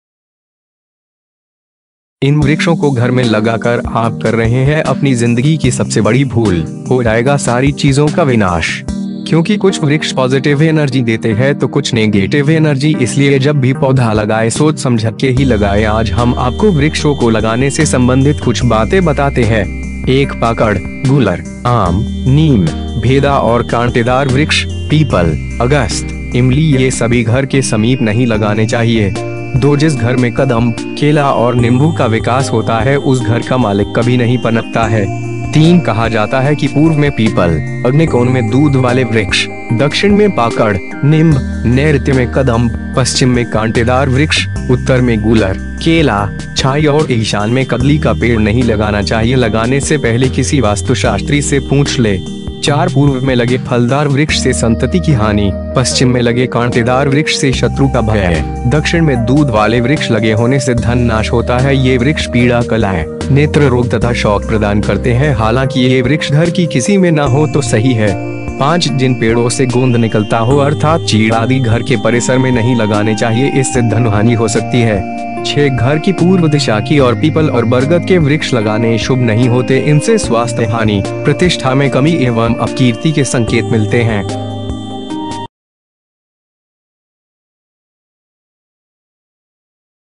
इन वृक्षों को घर में लगाकर आप कर रहे है अपनी जिंदगी की सबसे बड़ी भूल ,हो जायेगा सारी चीजों का विनाश
for mouth watering Indian Food Recipes इन वृक्षों को घर में लगाकर आप कर रहे है अपनी जिंदगी की सबसे बड़ी भूल ,हो जायेगा सारी चीजों का विनाश

क्योंकि कुछ वृक्ष पॉज़िटिव एनर्जी देते है तो कुछ नेगेटिव एनर्जी इसलिए जब भी पौधा लगाए सोच समझके ही लगाए आज हम आपको वृक्षो को लगाने से संबंधित कुछ बाते बताते है।

1 पाकड़,गूलर ,आम ,नीम ,भेदा और कांटेदार वृक्ष , पीपल, अगस्त, इमली ये सभी घर के समीप नहीं लगाने चाहिए।

2 जिस घर में कदम्ब ,केला और निम्बू का विकास होता है उस घर का मालिक कभी नहीं पनपता है।

3 कहा जाता है की पूर्व में पीपल ,अग्निकोण में दूध वाले वृक्ष ,दक्षिण में पाकड़ ,निम्ब ,नैऋत्य में कदम्ब, पश्‍चिम में कांटेदार वृक्ष,उत्तर में गूलर ,केला ,छाई और ईशान में कदली का पेड़ नहीं लगाना चाहिए लगाने से पहले किसी वास्तुशाष्त्री से पूंछ ले।

4 पूर्व में लगे फलदार वृक्ष से संतति की हानि, पश्चिम में लगे कांटेदार वृक्ष से शत्रु का भय, दक्षिण में दूधवाले वृक्ष लगे होने से धन नाश होता है ये वृक्ष पीड़ा कलह , नेत्ररोग तथा शोक प्रदान करते हैं हालाँकि ये वृक्ष घर की किसी में ना हो तो सही है।

5 जिन पेड़ो से गोंद निकलता हो अर्थात चीड़ आदि घर के परिसर में नहीं लगाने चाहिए इससे धनहानि हो सकती है।

6 घर की पूर्व दिशा की और पीपल और बरगद के वृक्ष लगाने शुभ नहीं होते इनसे स्वास्थ्य हानि ,प्रतिष्ठा में कमी एवं अपकीर्ति के संकेत मिलते हैं।